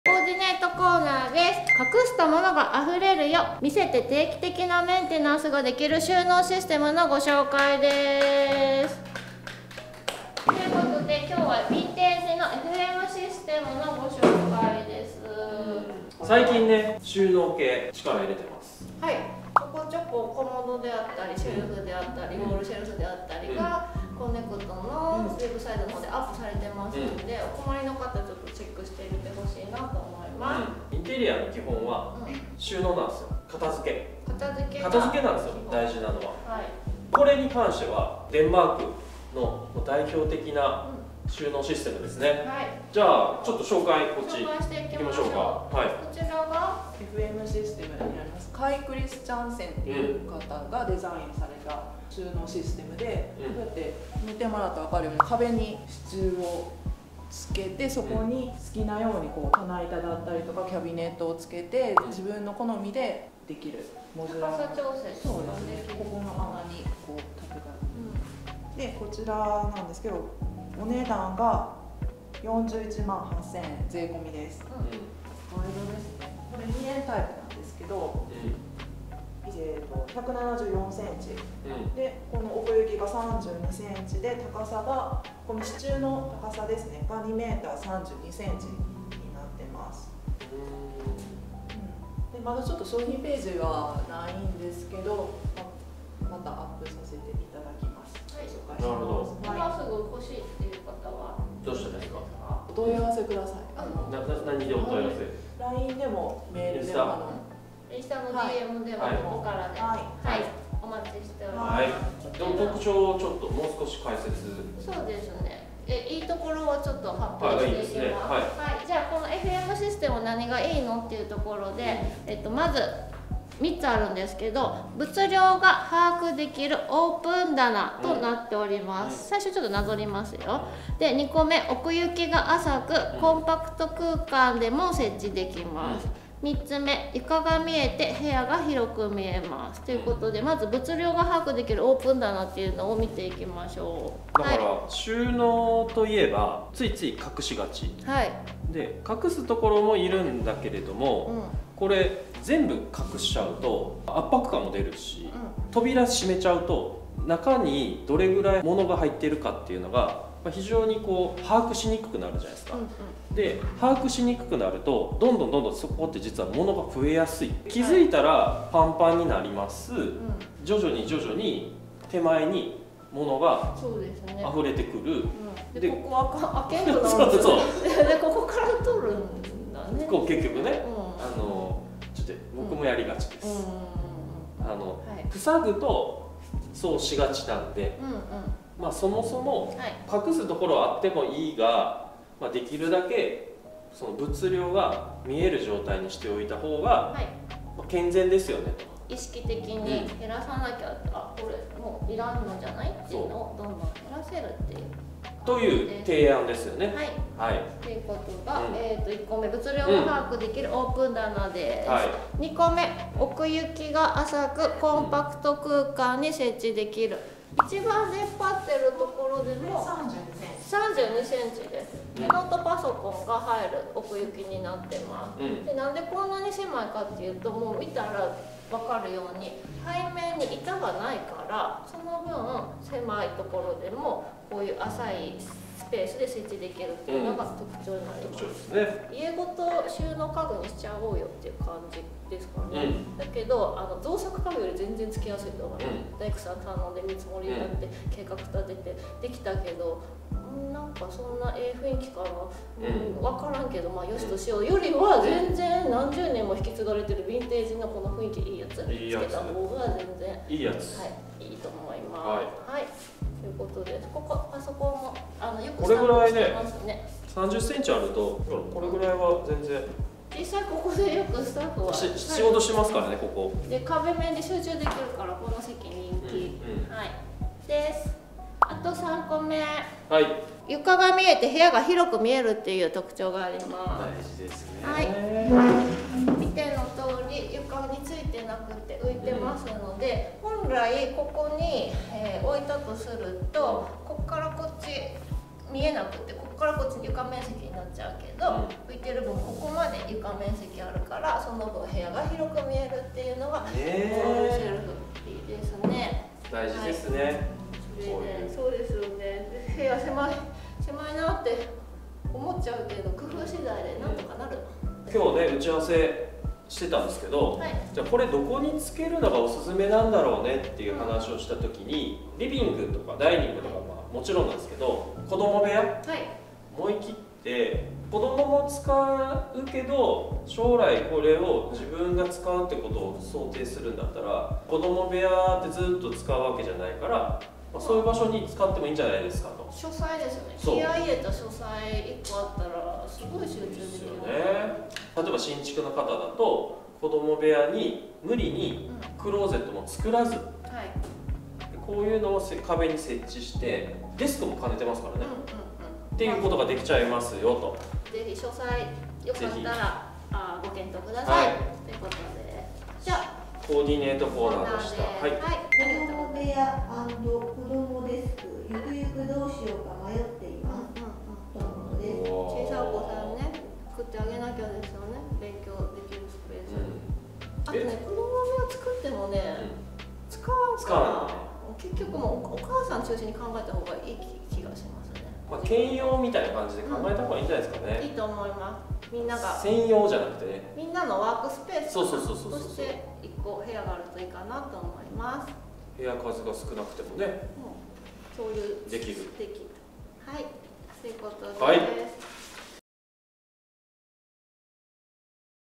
コーディネートコーナー｜おすすめヴィンテージ収納のご紹介
今回は、おすすめヴィンテージ収納をご紹介しました。

聞きづらかったり、画面が揺れることがありますがご了承ください。

■重要ポイント☝🏻✨
00:30　インテリアの基本は収納
01:39　ご購入方法
01:58　FMシステムの良いところ
03:09　見せる収納の良さ
03:30　ポイント
04:24　パソコンも置ける
04:39　床が広く見える
05:18　おすすめの使用場所

動画の感想はコメント欄へ✍🏻
ご質問等あれば下記よりお願いいたします。

■CONNECT

北欧インテリアを取り扱っているWEBショップです🛋️

営業日：月〜金（休日：土日祝）